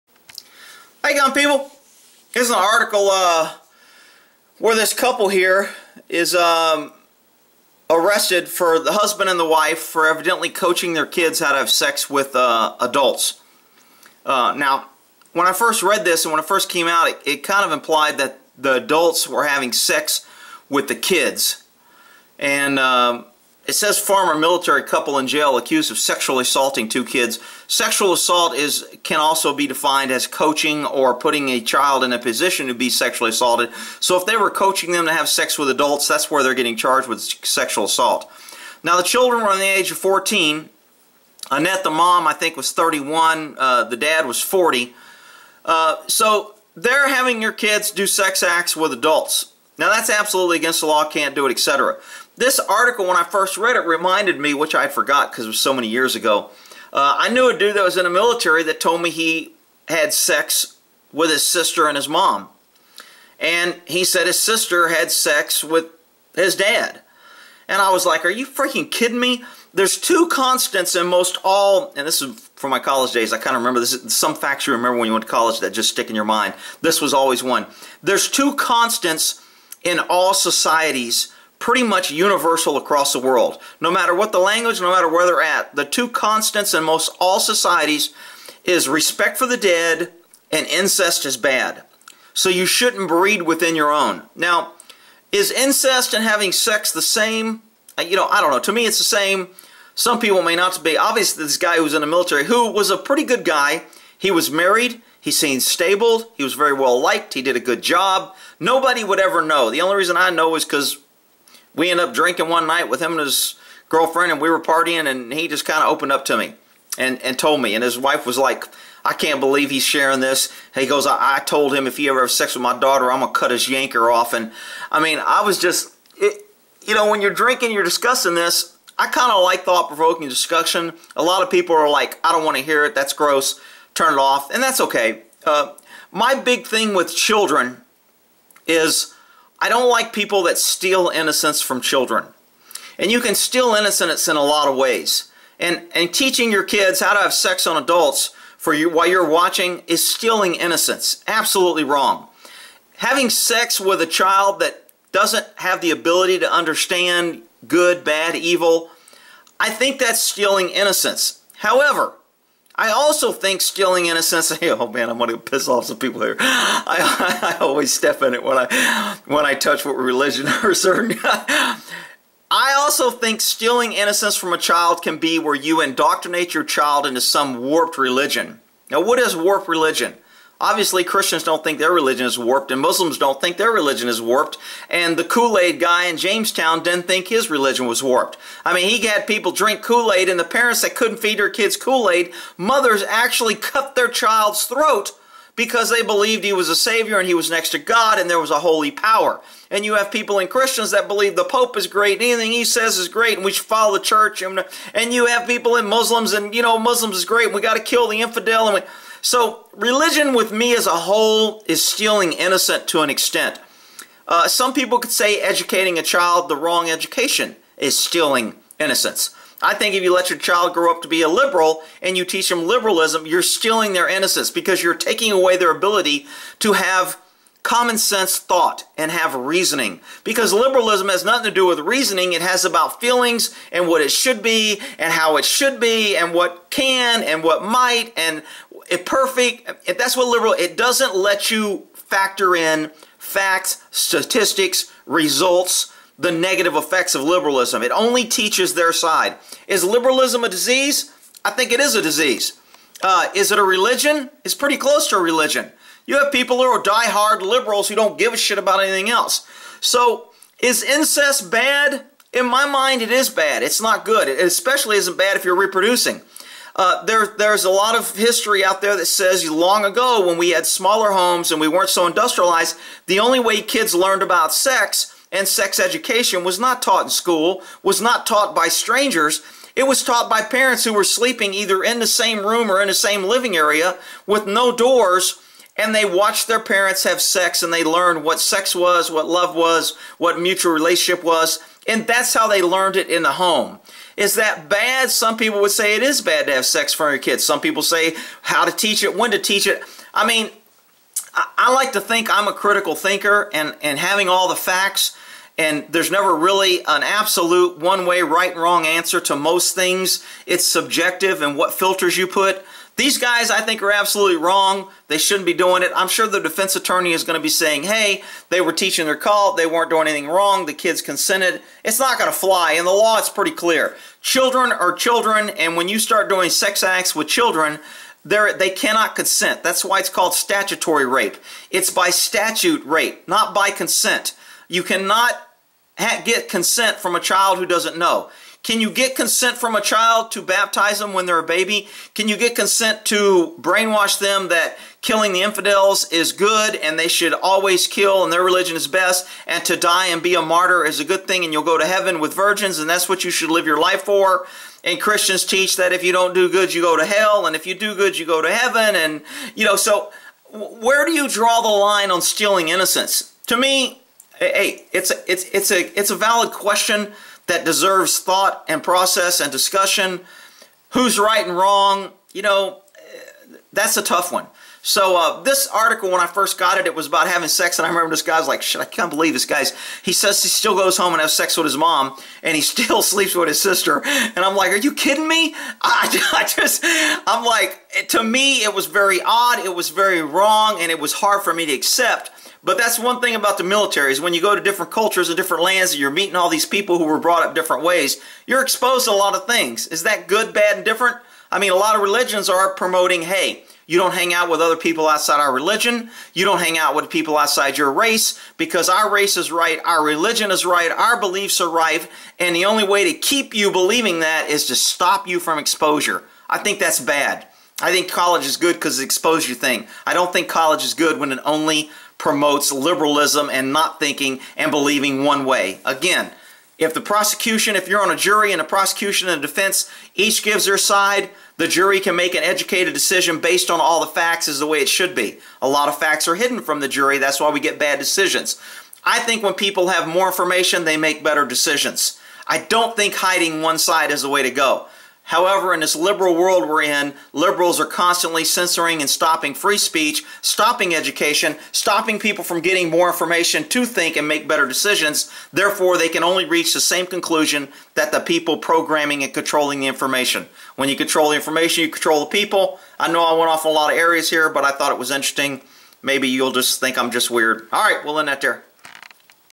Incest Religion Government & Liberalism - What Is Normal Stealing Innocent From Children
These parents wanted their kids to have sex with adults and the Kids consented is that against the Law? NOTE: MY TWITTER IS  @Horsyguy